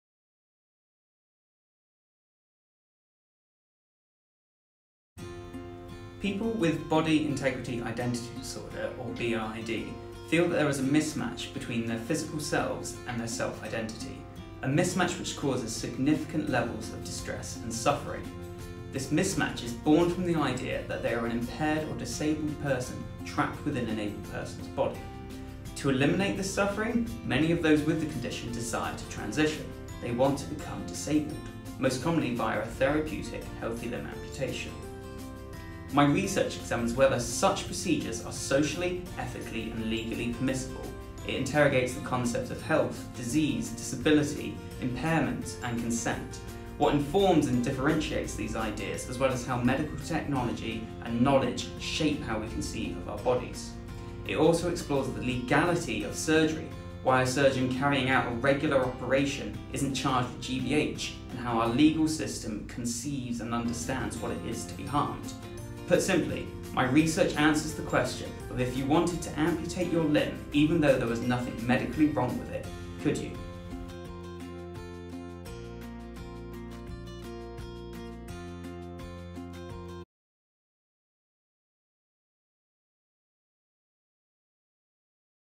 Richard Gibson, PhD Doctoral Candidate, on Body Integrity Identity Disorder.
Richard Gibson is currently undertaking a School of Law-funded PhD at the Centre for Social Ethics and Policy, based at The University of Manchester. Richard undertakes research in Philosophical Bioethics: specifically focusing on the ethical, legal, and social implication of elective amputation in cases of Body Integrity Identity Disorder.